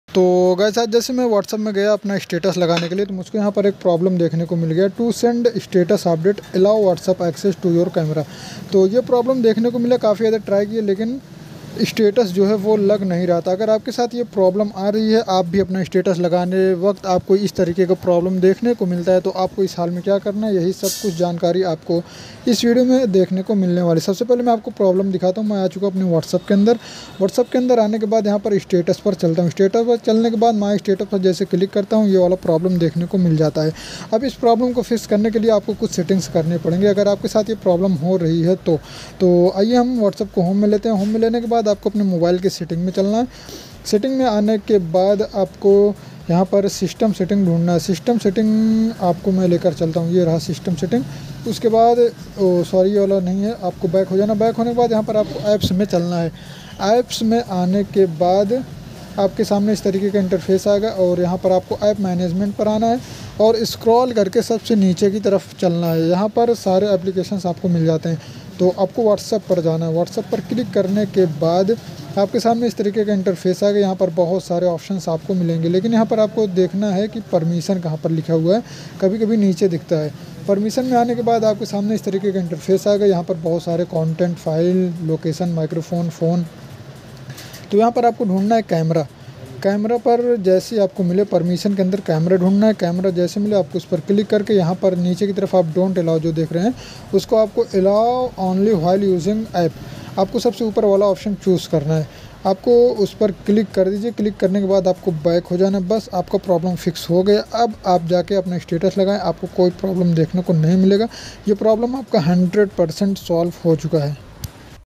to send a status update allow WhatsApp access to your camera problem fix ! WhatsApp status nahi lag
to send a status update allow WhatsApp access to your camera problem fix ! WhatsApp status nahi lag raha